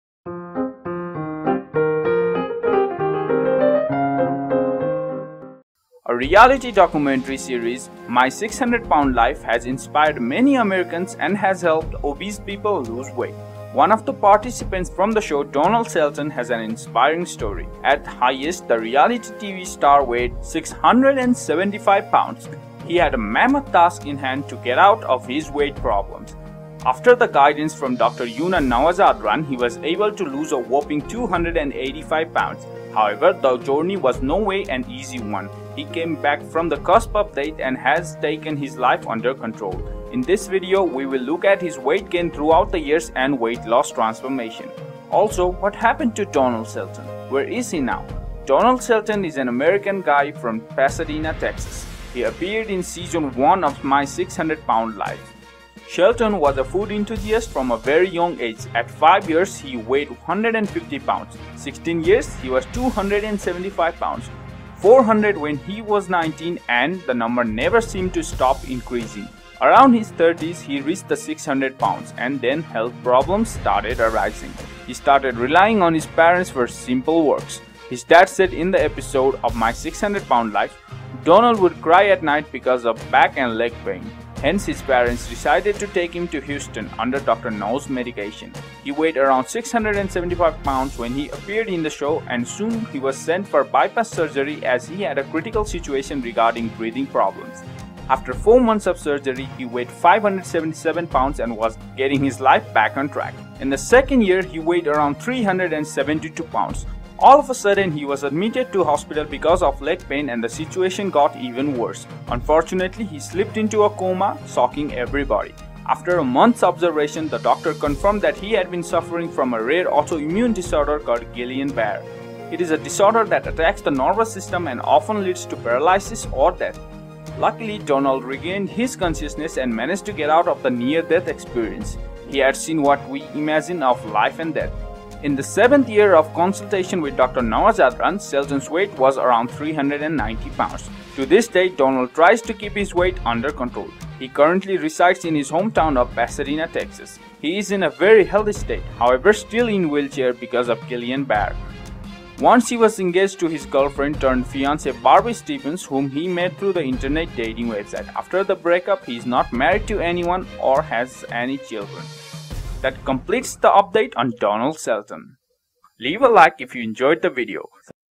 What happened to My 600-lb life donald shelton..?
A reality documentary series "My 600lb Life"  has inspired many Americans and has helped obese people lose weight.

One of the participants from the show Donald Shelton has an inspiring story.

At highest the reality tv star weighed  675 pounds.

He had a mammoth task in hand to get out of weight problems.

After the guidance from Dr. Younan Nowzaradan, he was able to lose a whopping 285 pounds.

However, the journey was no way an easy one.

He came back from the cusp of death and has taken his life under control.

In this video, we will look at his weight gain throughout the years and weight loss transformation.

Also what happened to Donald Shelton? Where is he now?

Donald Shelton is an American guy from Pasadena, Texas.

He appeared in season 1 of My 600-lb Life.

Donald Shelton was a food enthusiast from a very young age.

At 5 years,  he weighed 150 pounds,16 years he was 275 pounds, 400 when he was 19, and the number increased simultaneously.

Around his 30s, he reached around 600 pounds, and then health problems started arising.

He started relying on his parents for simple works.

His dad said in the episode of "My 600 lb life" Donald would cry at night because of back and leg pain.

Hence his parents decide to take him to Houston, under Dr. Now's medication.

He weighed around 675 pounds when he appeared in the show and soon he was sent for Bypass surgery as he had a critical situation regarding breathing problems.

After 4 months of surgery, he weighed 577lbs and was getting his life back on track.

In the second year, he weighed around 372 pounds.

All of a sudden he was admitted to hospital because of leg pain and the situation get even worse.

Unfortunately, he slipped into a coma shocking everybody.

After a month's observation, the doctors confirmed that he had been suffering from a rare auto-immune disorder called Guillain-Barre.

It is a disorder that attacks the nervous system and often leads to paralysis or death.

Luckily, Donald regained his consciousness and managed to get out of the near-death experience.

He had seen what we imagine of life and death.

In the seventh year of consultation with Dr. Now Shelton's weight was around 390.

To this day Donald tries to keep his weight under control.

 He currently resides in his hometown of Pasadena, Texas.

He is in a very healthy state, however still in Wheelchair because of Guillain Barre.

Once he was engaged to his girlfriend turned fiancee Barbie Stephens whom he met through the internet dating website.

After the breakup, he is not married to anyone or has any children.

This is all update we have got on reality TV star.